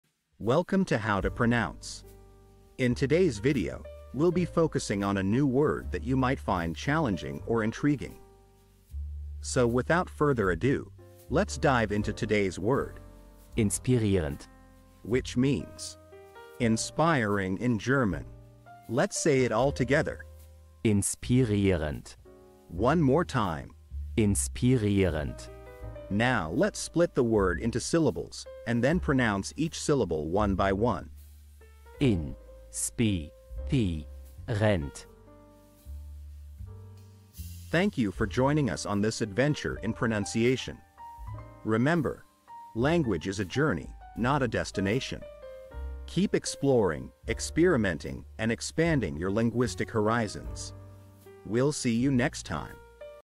How to pronounce Inspirierend
🌍 Learn a new language now Master the pronunciation of 'Inspirierend' Inspirierend 📖 - which means : Inspiring in German 📝 - with this quick and clear tutorial by How To Pronounce!

In this video 📽️, we'll guide you through the correct pronunciation of 'Inspirierend' in German, using native speaker examples 🗣️👂 and easy-to-follow phonetic breakdowns 🎵. Whether you're learning German for travel ✈️, business 💼, or just for fun 🎉, our guide will help you perfect your pronunciation in no time ⏰.